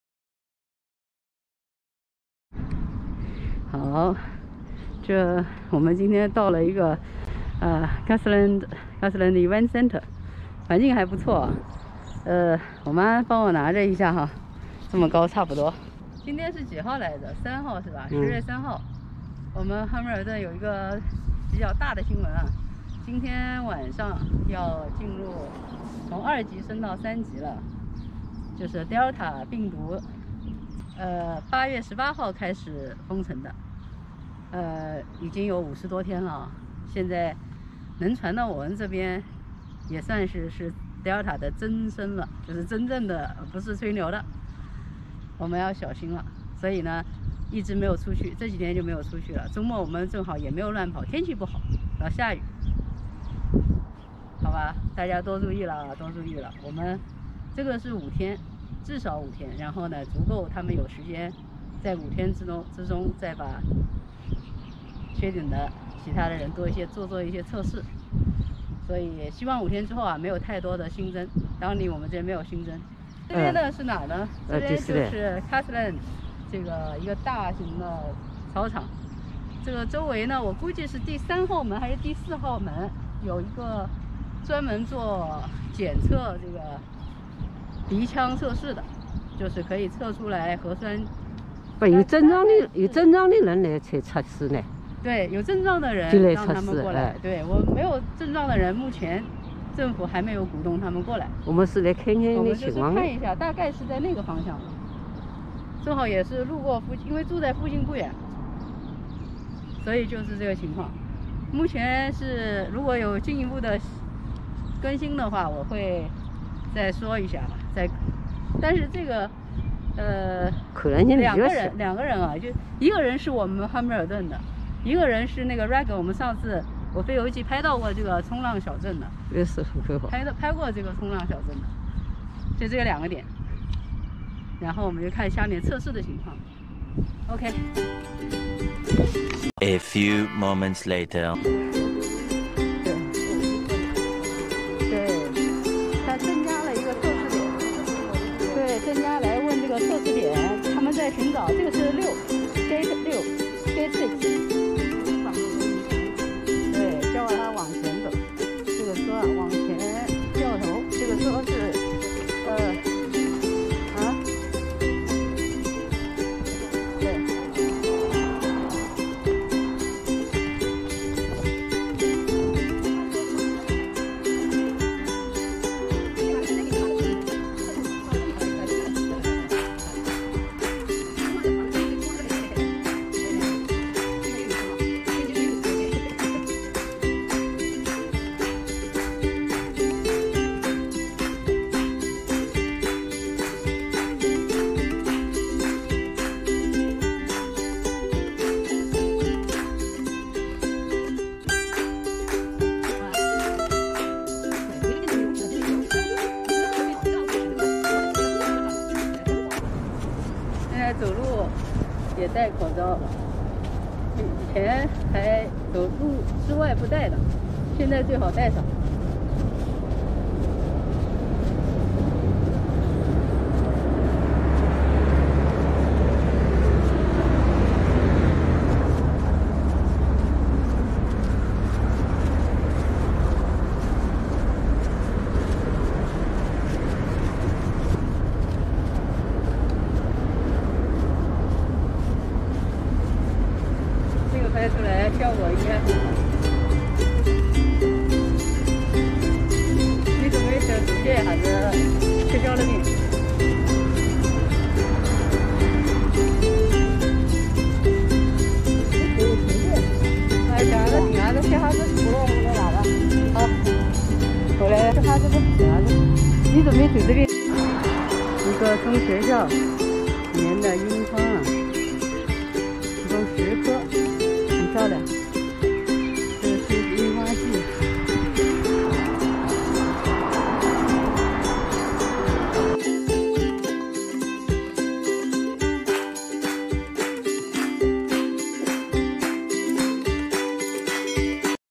惊: 德尔塔真的来了到汉密尔顿了 2例已确诊可能增加 晚手机紧急警报响起 今夜怀卡托多地防疫Alert升3级 节奏太快 我和飞妈下午去买菜呆家抗疫了 路过樱花看习惯了就好-Fay飞游记新西兰130集
3 Oct周日 该来的总该来 新西兰汉密尔顿今天下午德尔塔疫情出现确诊 德尔塔终于侵入Hamilton市及近邻 怀卡托已有2人确诊 一例在Raglan另一例在Hamilton但有可能测试后会有增加 我的手机晚7:45pm紧急警报突然拉响着实吓人一跳 官宣10月3日晚防疫Alert二升三级至少五天 还好我和飞妈下午步行去买点儿菜准备抗疫了 各大教授称除了多打疫苗已无它法 路过的樱花很美 我路过看到美的丑的原来习惯了就好-飞游记新西兰130集  注-google或YT可搜索“飞游记 Raglan” "飞游记 Hamilton"